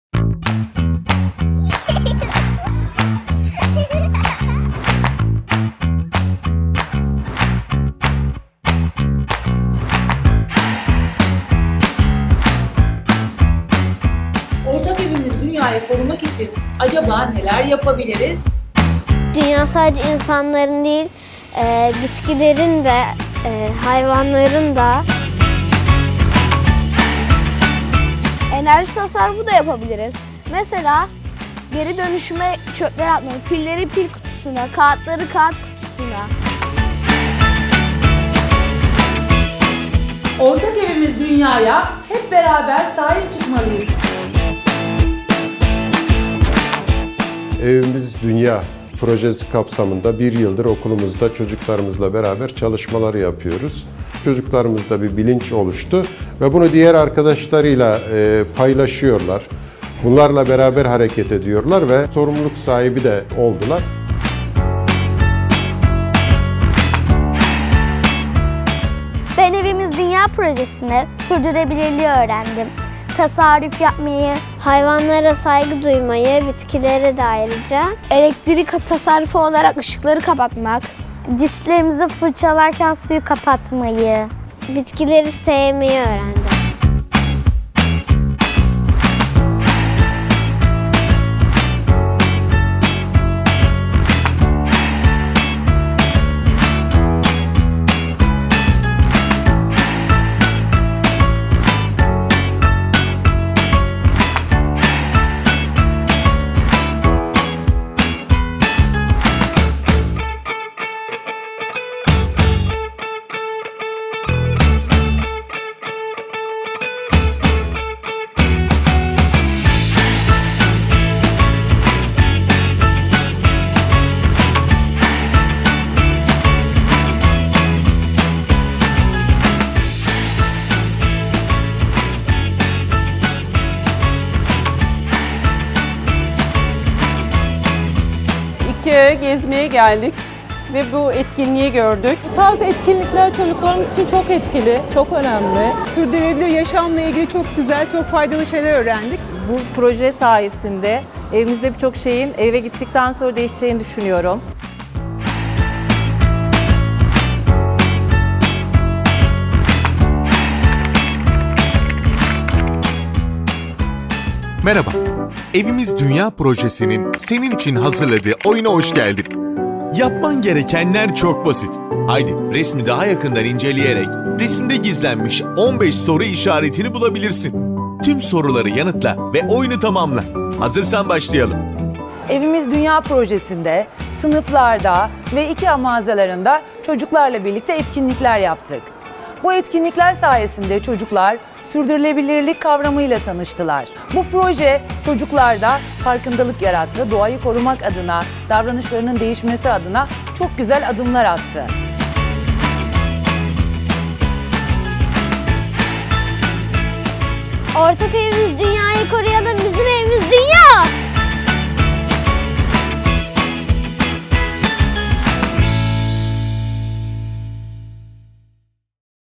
Evimiz Dünya Projesi
IKEA Türkiye ve Milli Eğitim Bakanlığı (MEB) iş birliğinde başlayan Evimiz Dünya Projesi ile çocuklara sürdürülebilirlik ve tüketim alışkanlıklarıyla ilgili farkındalık kazandırılmasını hedefliyoruz.. Proje IKEA Türkiye mağazalarındaki poşetlerin satışlarından elde edilen gelirle hayata geçiriliyor.